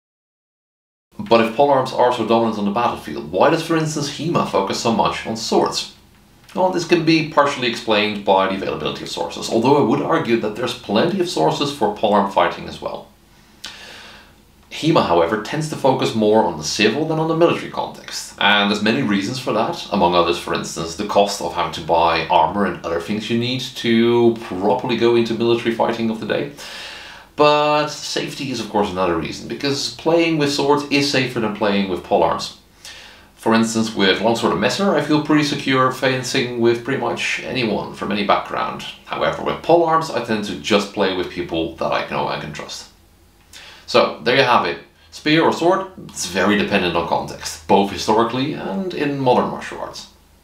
Spears, swords and context 2: why does HEMA focus so much on swords and not spears?
A few days ago I saw this illustration by Drawing Armoury pop up . In the comments, people immediately started arguing whether swords or spears are better, so I figured it's about time for a short video on this subject. In this one, I try to give a short answer to the question why HEMA in majority focusses more on swords and less on polearms.